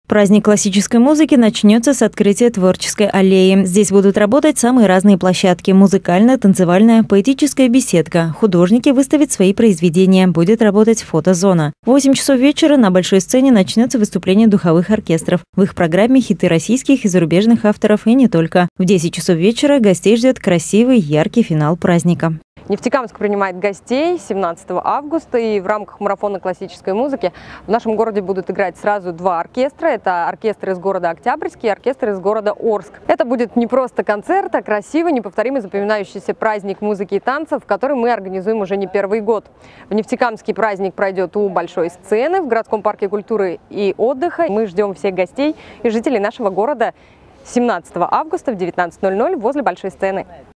В Нефтекамске сыграют оркестры из Оренбурга и Орска
В Нефтекамске пройдет «Марафон классической музыки». Уникальный вечер под открытым небом состоится 17 августа в городском парке культуры.